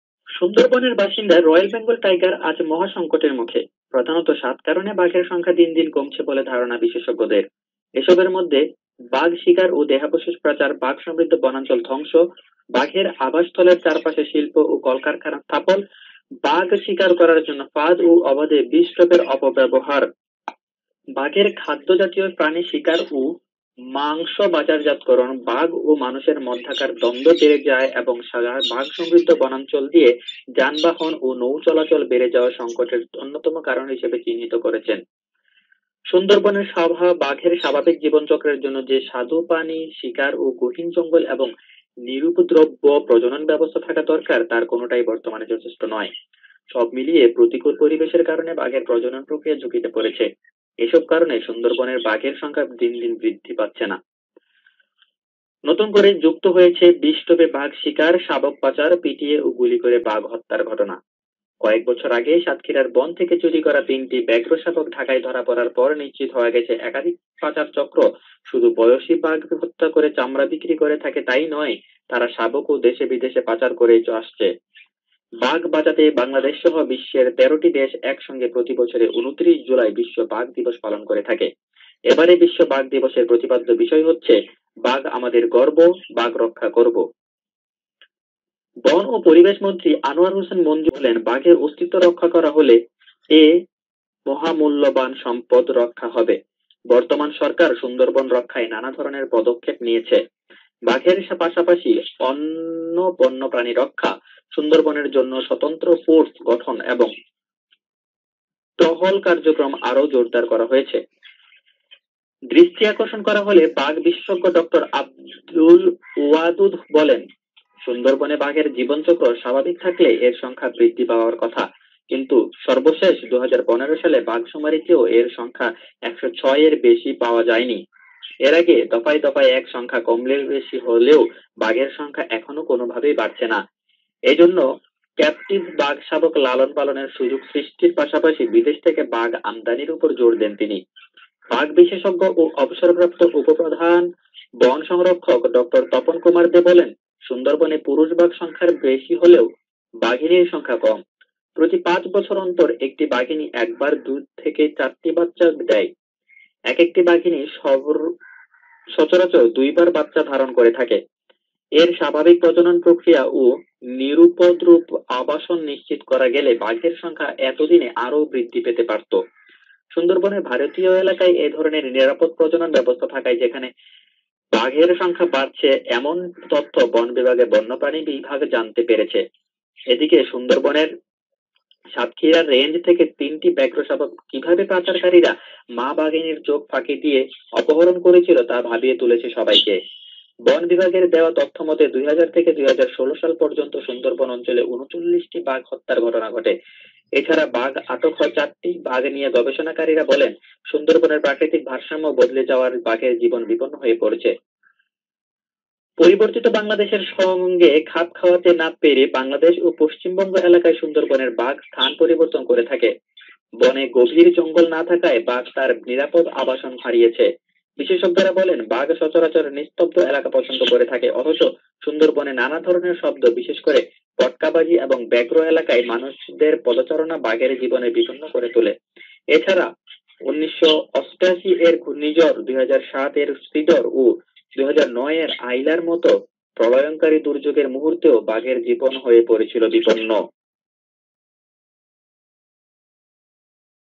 যে সাতটি কারনে সুন্দরবনের রয়েল-বেঙ্গল-টাইগার আজ হুমকির মুখে.....
shati karone aj shondor bon er royal bangol taigar aj homkir mukhomukhi.